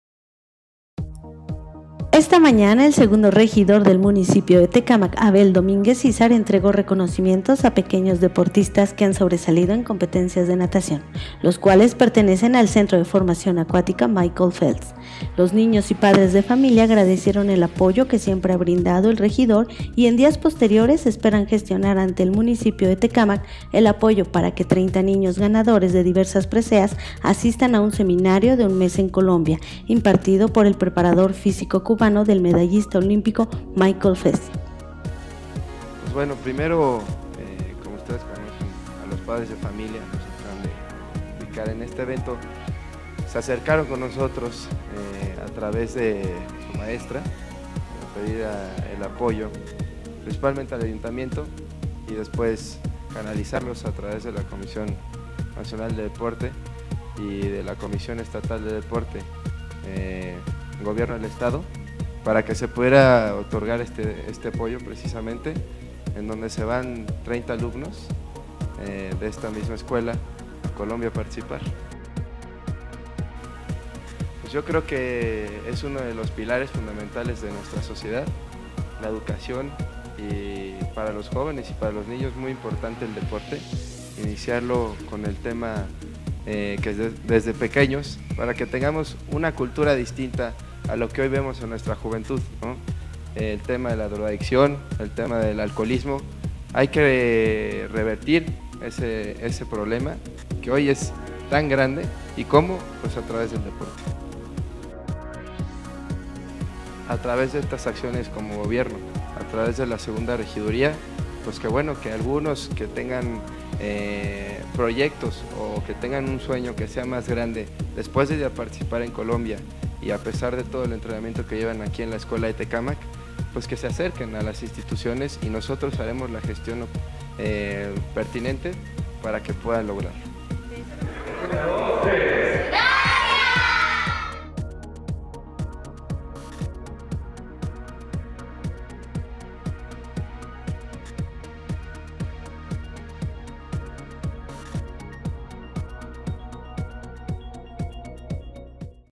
Abel Domínguez Izar entrega reconocimiento a niños deportistas
Tecámac, Estado de México.- El segundo Regidor del municipio de Tecámac;  Abel Domínguez Izar entrego reconocimientos a a pequeños deportistas, que han sobresalido en competencias de natación, los cuales pertenecen al Centro de Formación Acuático Michael Phelps, ubicado en la col. Ejidos.
Los niños y padres de familia agradecieron el apoyo que siempre ha brindado el regidor cuando han participado en competencias a nivel local, estatal y nacional y es dias posteriores esperan gestionar ante el muniicpio de Tecámac el apoyo para que 30 niños ganadores de diversas preseas asistan a un seminario de un mes en colombia impartido por el preparador fisico cubano del medallista olimpico.